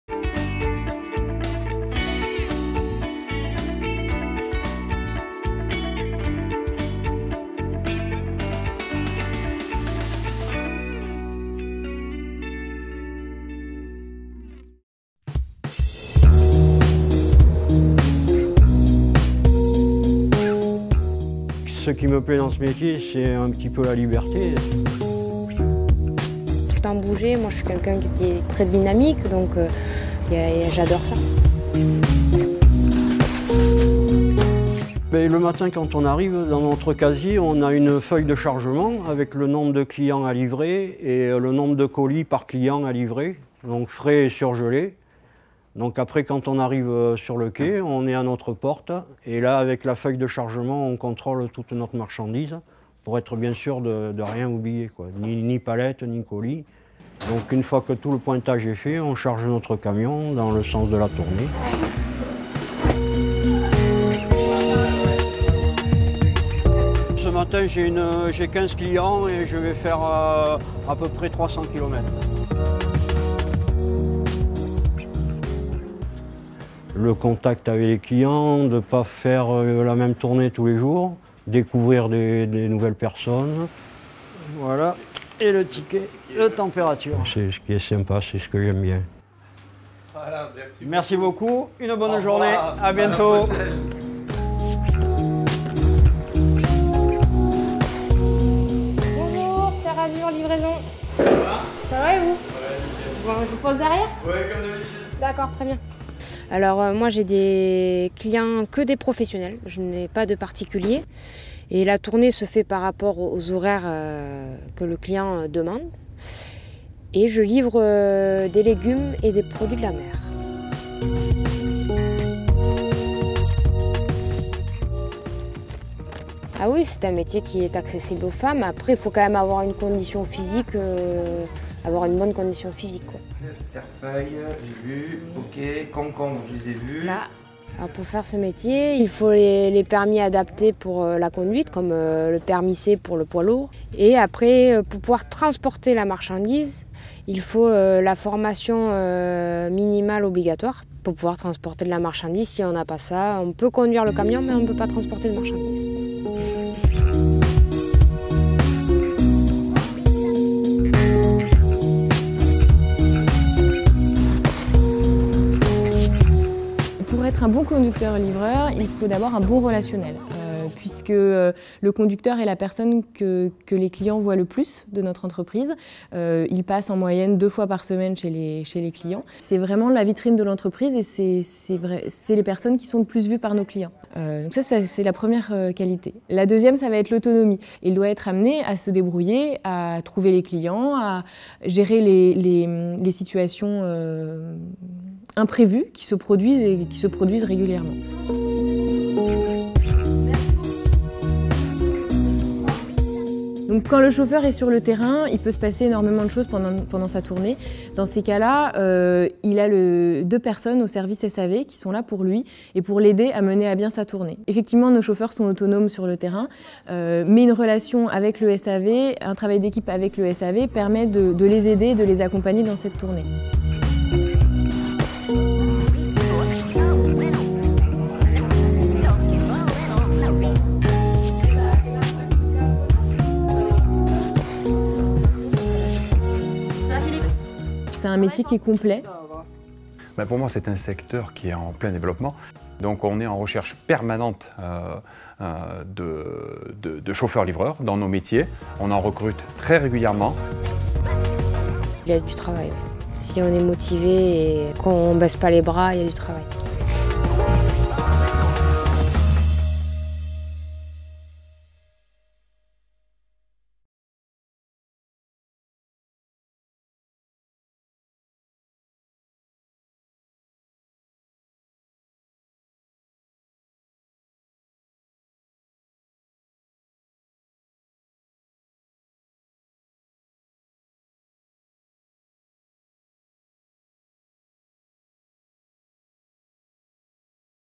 Le métier de conducteur.rice livreur.euse
Découvrez le métier de conducteur livreur / conductrice livreuse Poids Lourd, un métier qui bouge !
Le transport-logistique, et si c'était votre voie ?